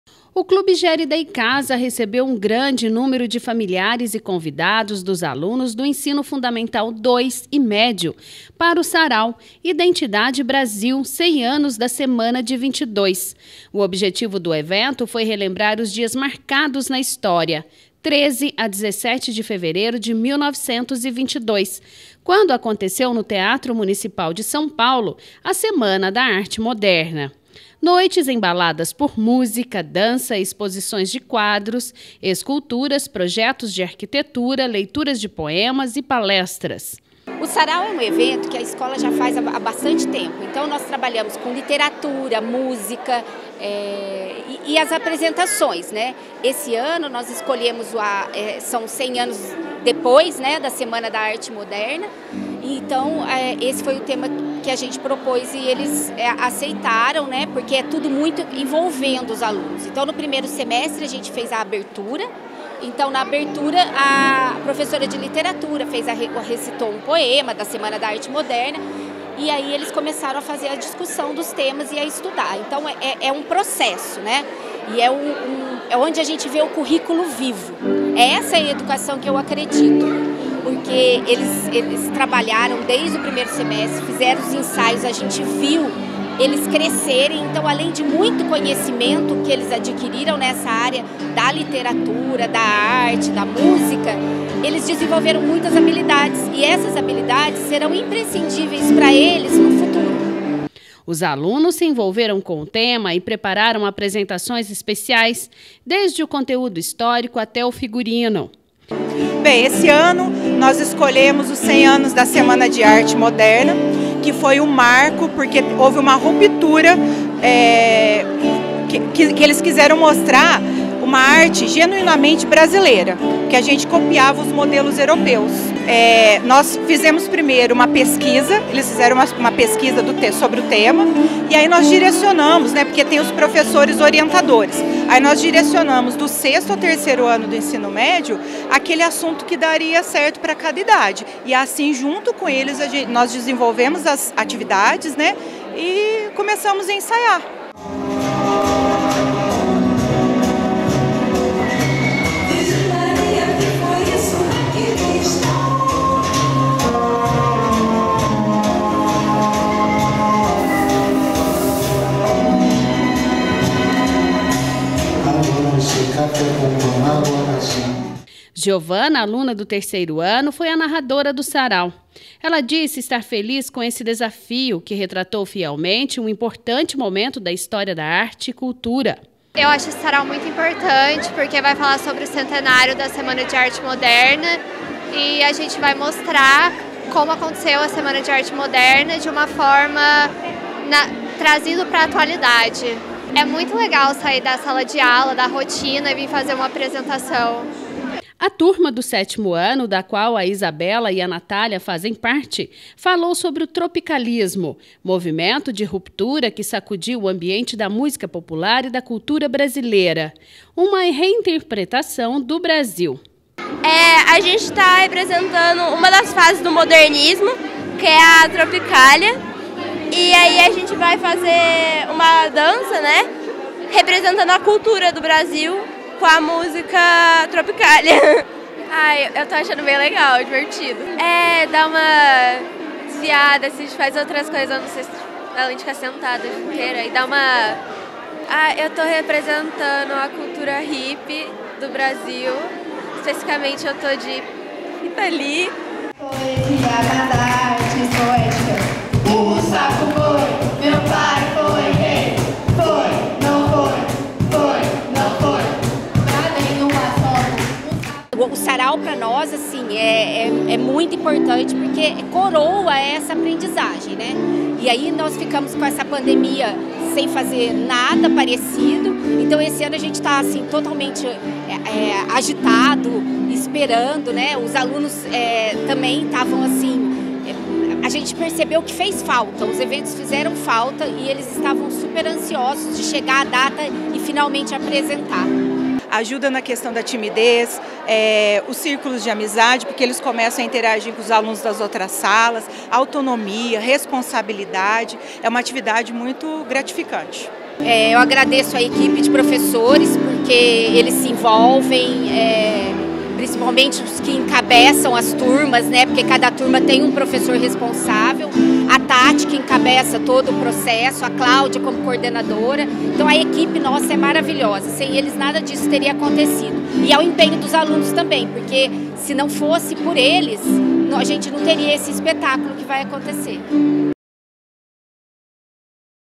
Colégio Junqueira realiza Sarau com o tema : 100 anos da Semana da Arte Moderna
O Colégio Junqueira realizou na última semana mais uma edição de um dos seus tradicionais eventos- o sarau 2022, que teve como tema: Os 100 anos da Semana da Arte Moderna.